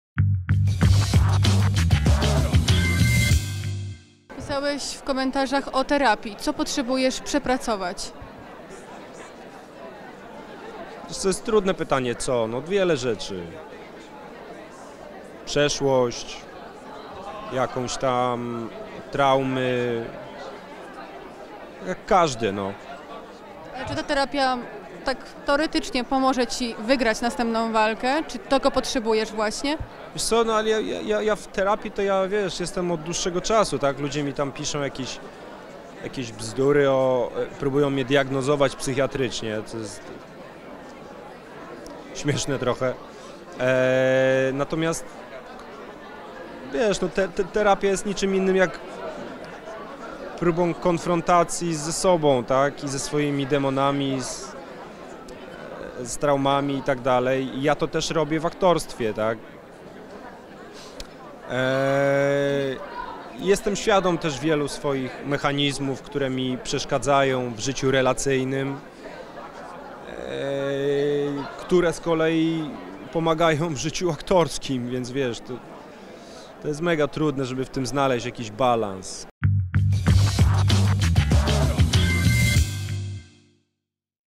Sebastian Fabijański szczerze o terapii. "Próbują mnie diagnozować psychiatrycznie"
Sebastian Fabijański nie ma teraz łatwego czasu. Aktor poniósł porażkę w sparingu podczas gali Fame MMA i musiał mierzyć się z ogromnym hejtem. W rozmowie z Plejadą celebryta opowiedział o walce ze swoimi demonami i terapii.